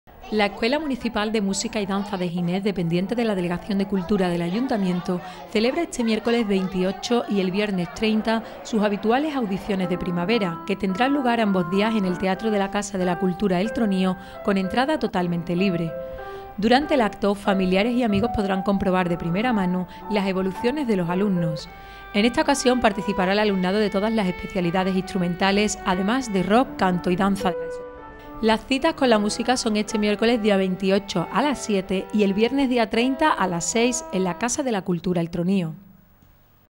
Miércoles y viernes, Audiciones de Primavera de la Escuela Municipal de Música y Danza de Gines
Será este miércoles día 28 a las 19.00 horas y el viernes día 30 a las 18.00 horas en la Casa de la Cultura "El Tronío" con entrada totalmente libre